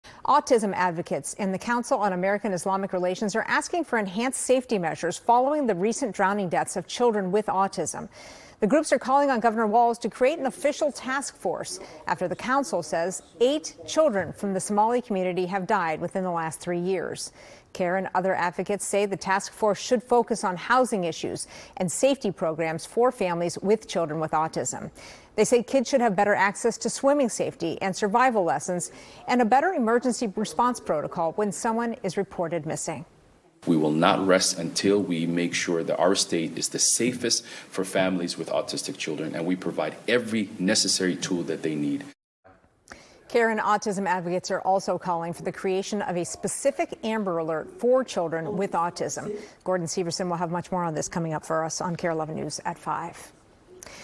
CAIR and Autism Advocates Urge Minnesota to Enhance Safety for Children with Autism
CAIR-MN and autism advocates are calling on Governor Tim Walz
 to take action following the tragic drowning deaths of several children with autism, including eight Somali children in the past three years. Together, they’re urging the creation of an official task force to address safety for children with autism, with a focus on housing support, water safety programs, and improved emergency response protocols.

“We won’t rest until Minnesota is a safe and supportive place for families with autistic children,” said CAIR. Advocates also hope to establish an autism-specific Amber Alert to aid in faster response times.